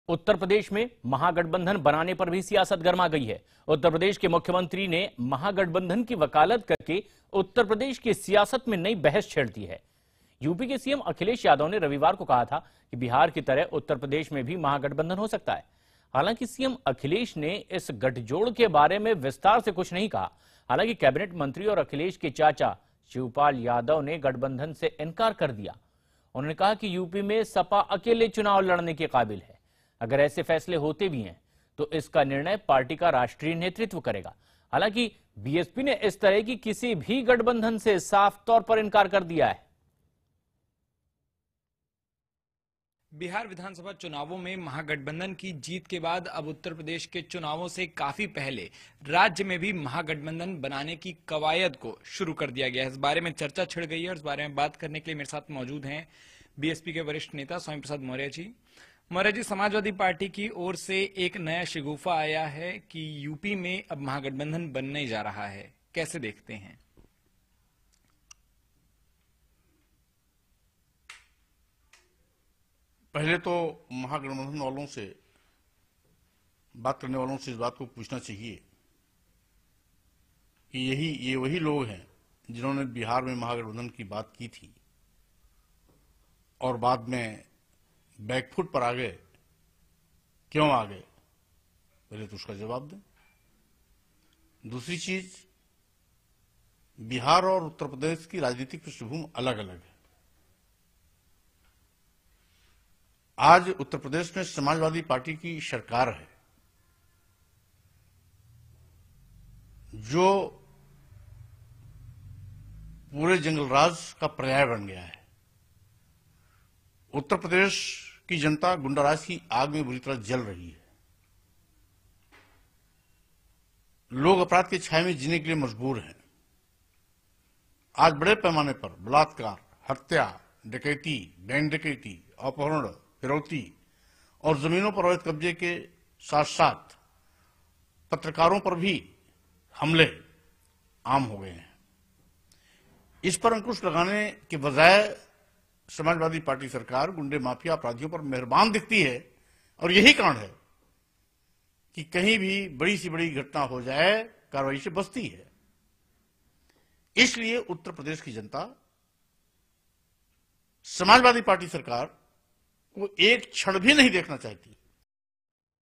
UP Me Nahi Banega Mahagathbandhan, BSP Ne SP Ke Sath Aane Se Mana Kiya
बिहार में महागठबंधन को बीजेपी के खिलाफ मिली जीत के बाद यूपी में भी महागठबंधन की अटकलें तेज हो रही थीं। एक तरफ जहां यूपी के मुख्यमंत्री अखिलेश यादव ने महागठबंधन की संभावना से इनकार ना करते हुए इसकी वकालत की थी वहीं बसपा ने इसे सिरे से नकार दिया है।